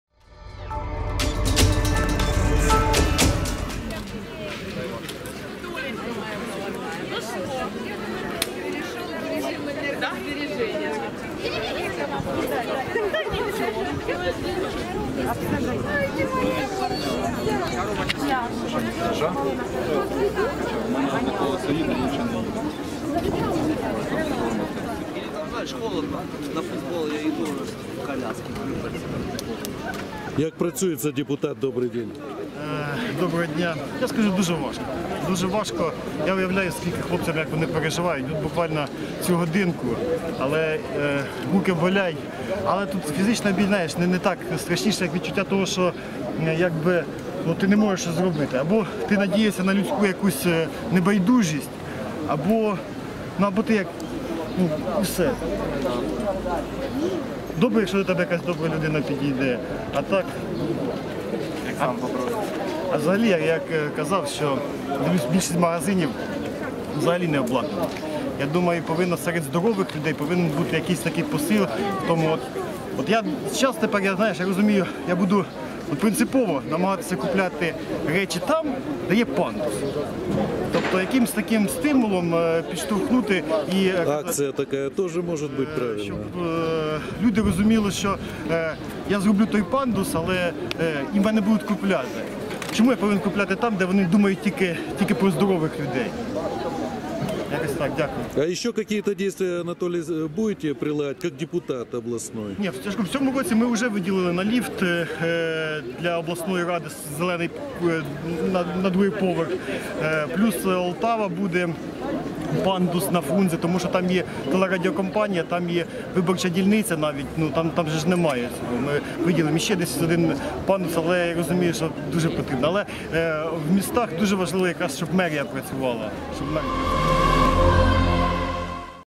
Полтава 6.5.2016 Анатолий Ханко: Дуже важко
Депутат областного совета на себе испытал неудобства которые испытывают на себе люди с ограниченными возможностями.